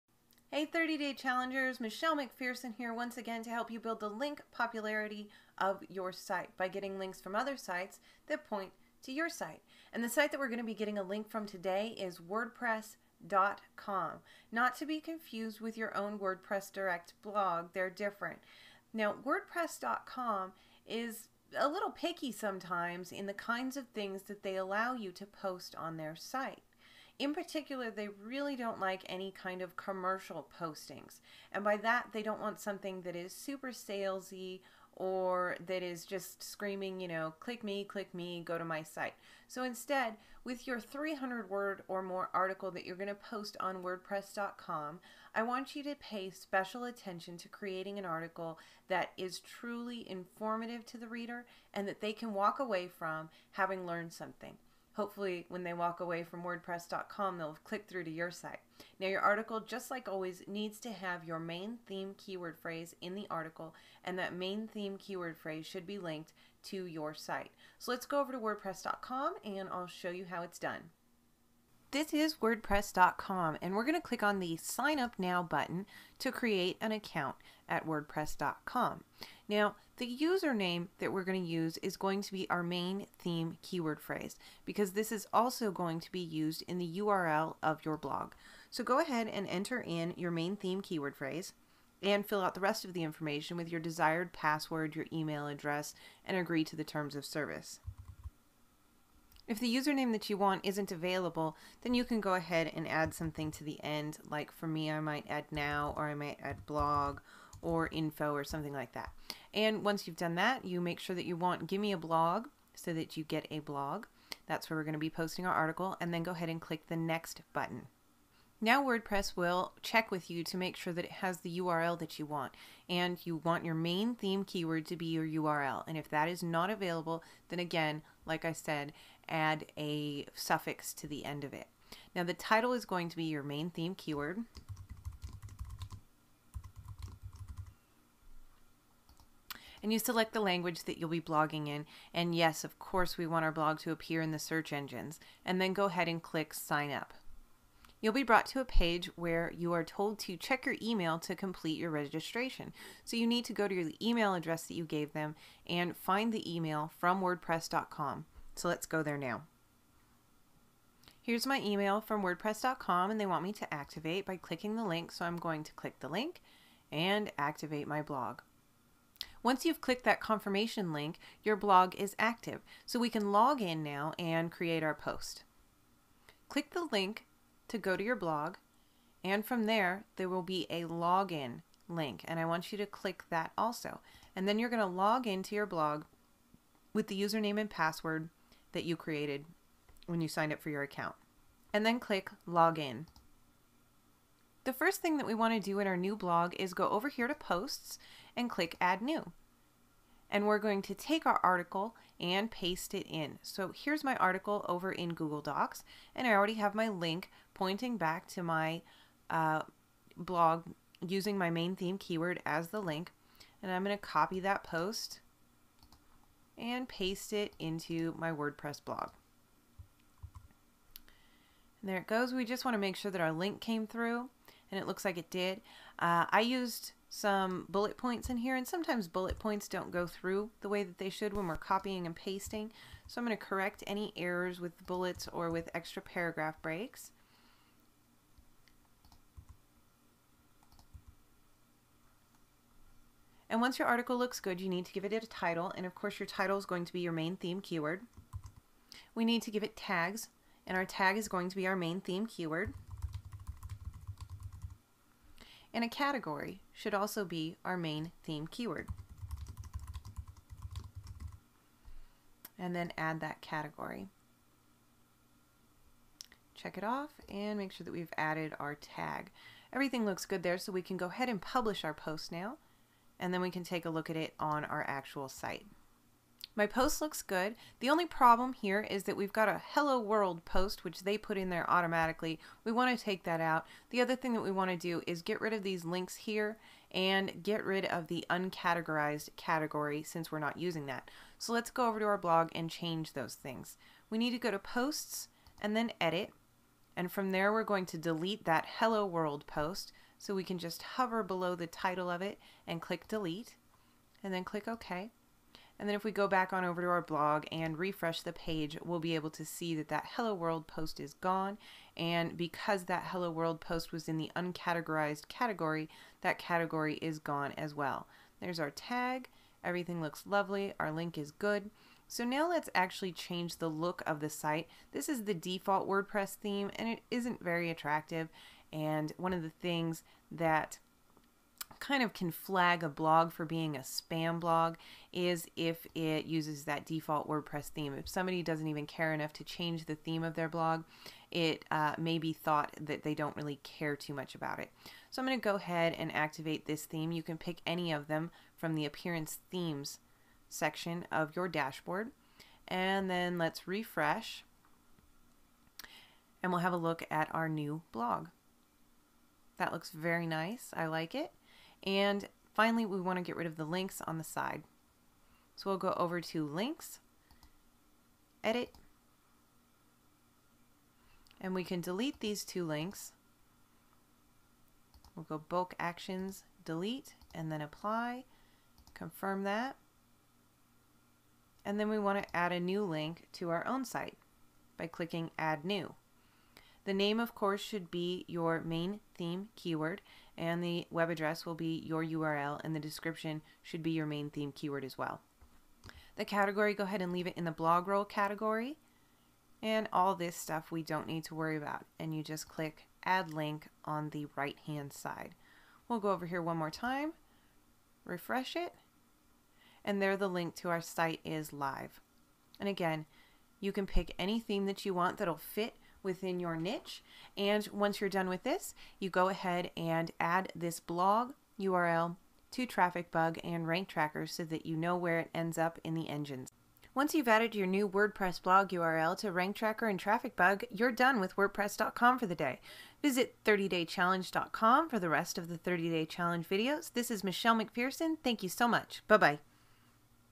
30DC Day 13 - Wordpress
Michelle shows you how to set up a Wordpress.com blog.